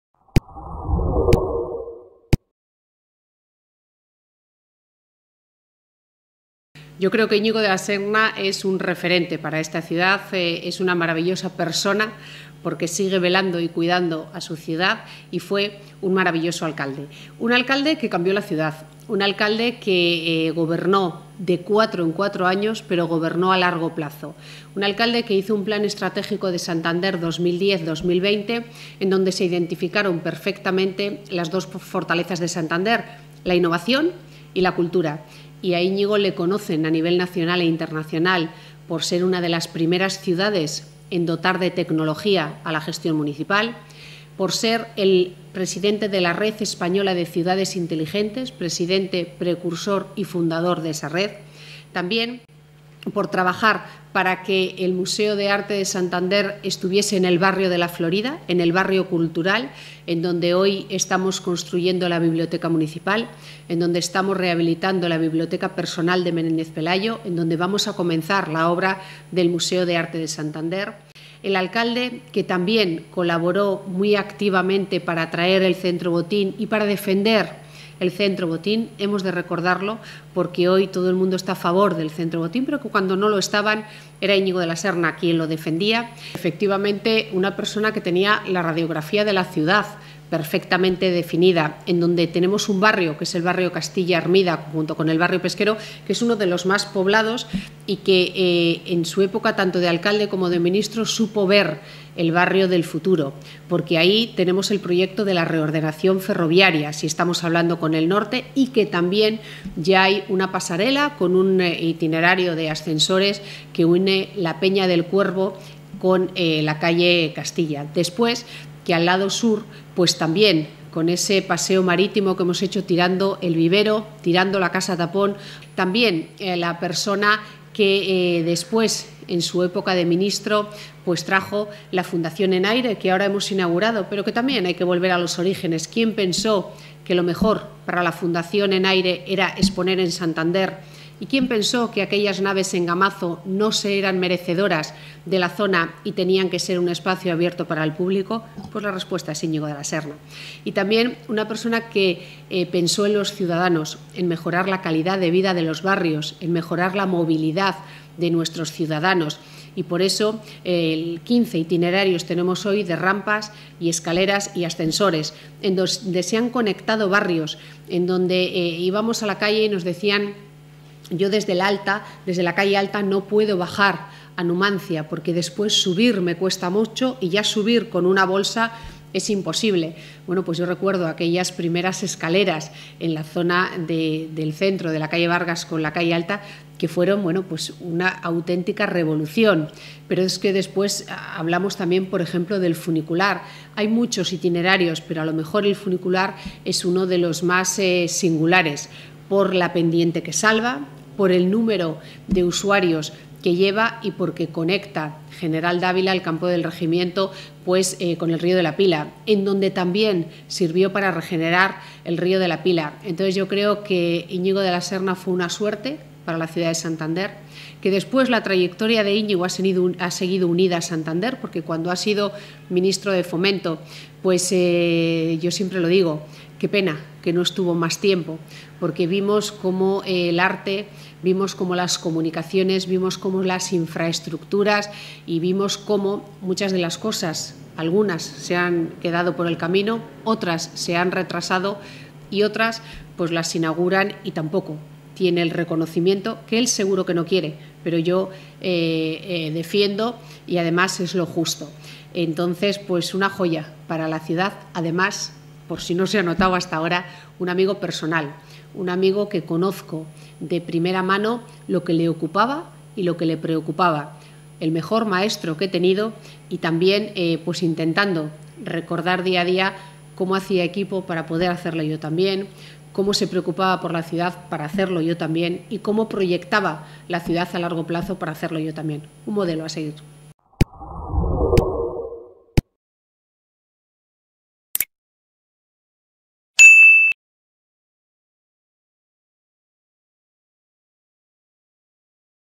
LEGADO DE IÑIGO DE LA SERNA EN EL AYUNTAMIENTO DE SANTANDER
Gema Igual, la actual Alcaldesa de Santander recuerda el legado dejado por Iñigo de la Serna en la Capital de Cantabria